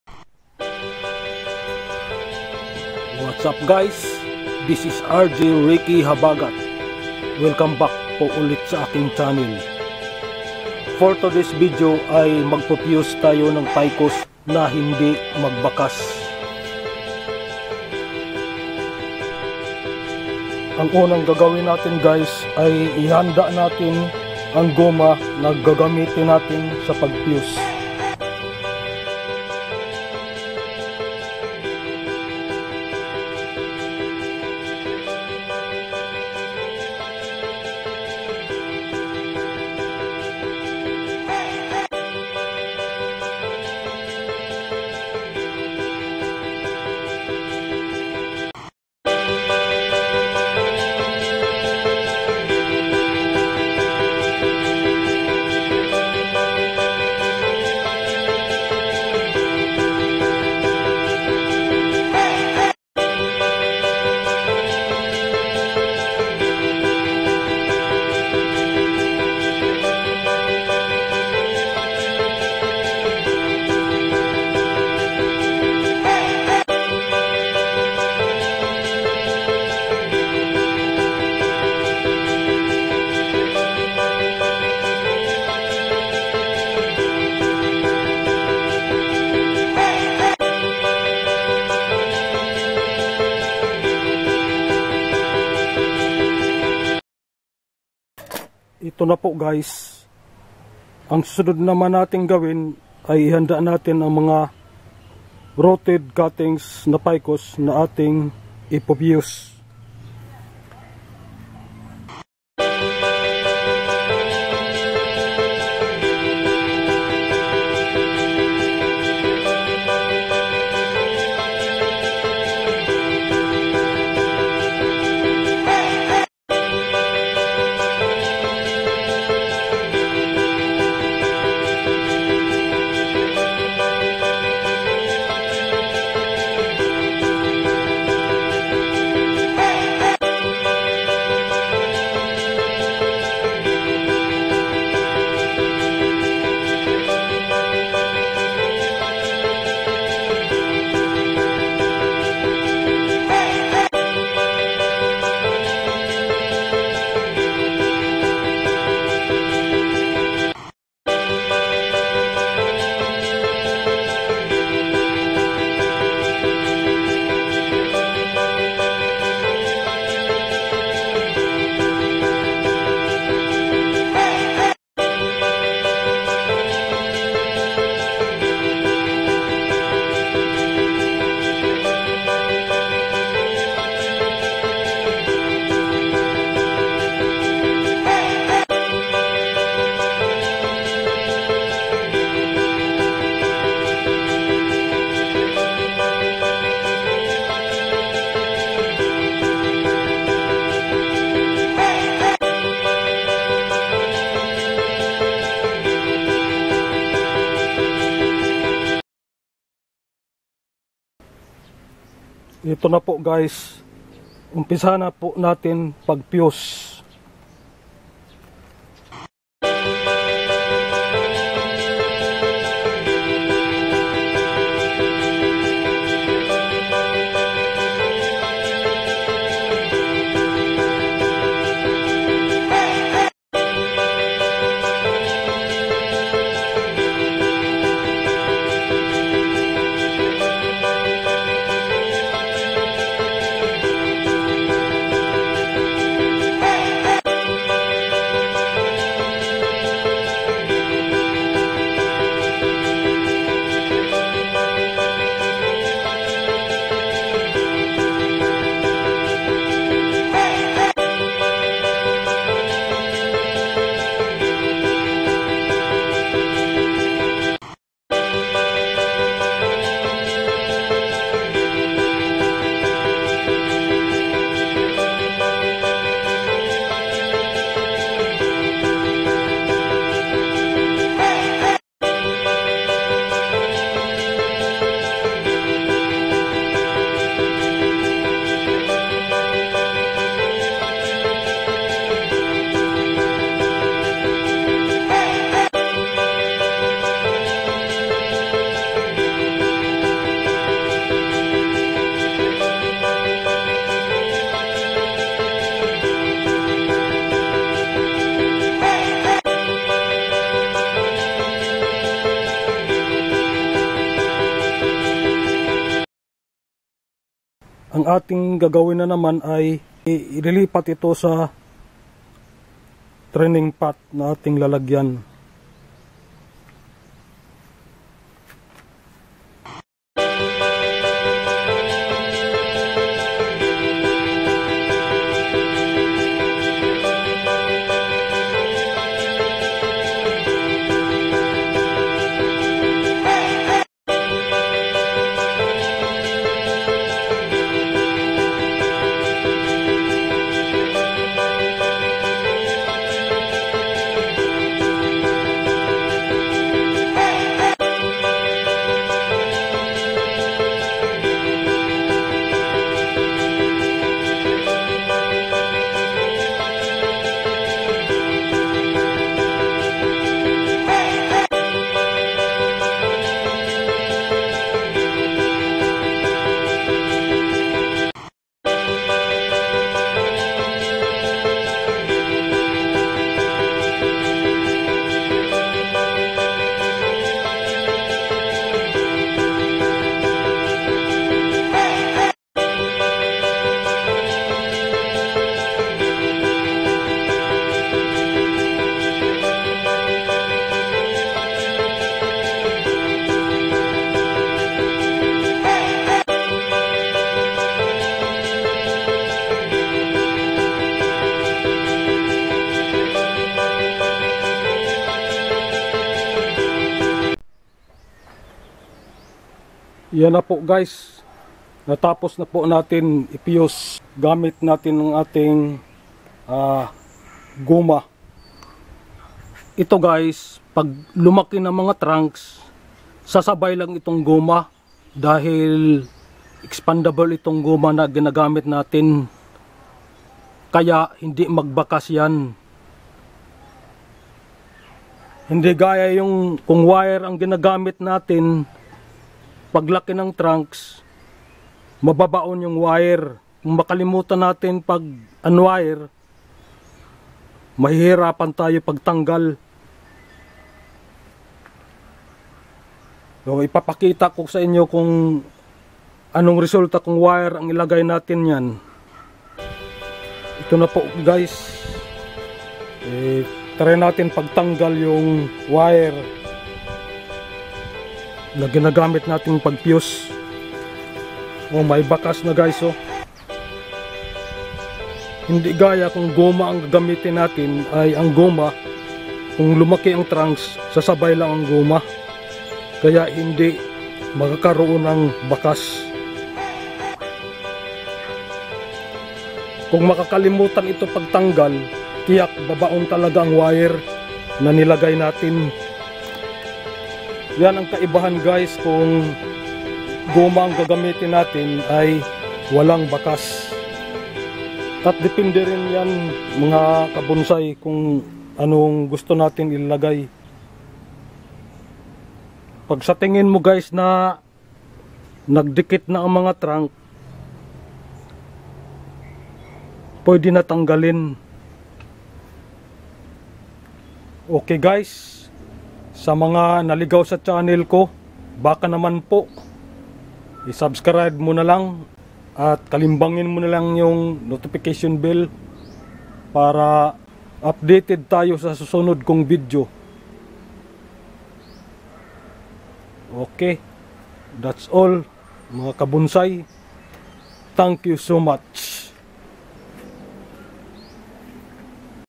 How to Fuse Ficus without any marking on the Trunks..
How to Fuse Ficus without any marking on the Trunks. Paano mag Fuse ng Ficus na hindi magbakas sa Trunks.